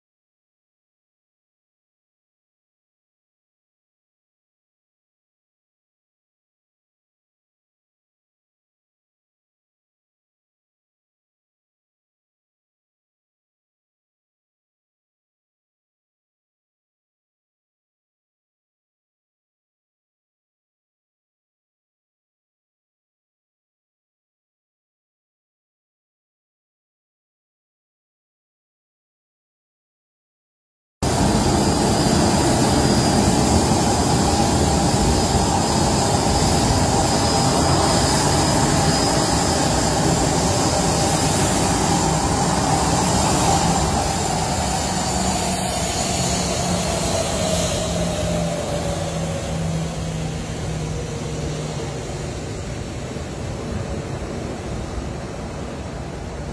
President Buhari Departs to Saudi Arabia for Economic Forum of the Future Investment Initiative
President Muhammadu Buhari departs for an official trip to the Kingdom of Saudi Arabia (KSA), to attend the Economic Forum of the Future Investment Initiative (FII) in Riyadh